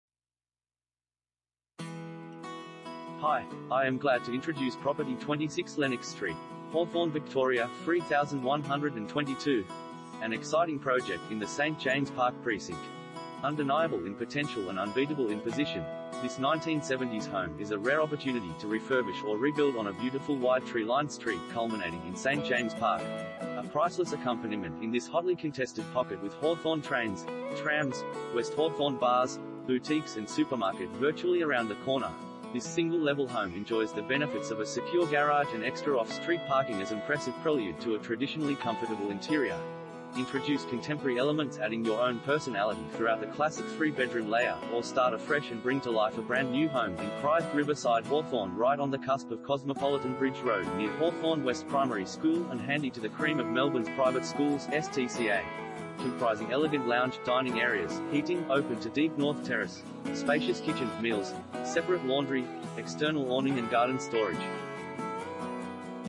26 Lennox Street, Hawthorn VIC 3122 | AuPropertyReport.Com
26 Lennox Street, Hawthorn VIC 3122
Property Type: house
Bedrooms: 3
Bathrooms: 3
Car Space: 2
An exciting project in the St James Park precinct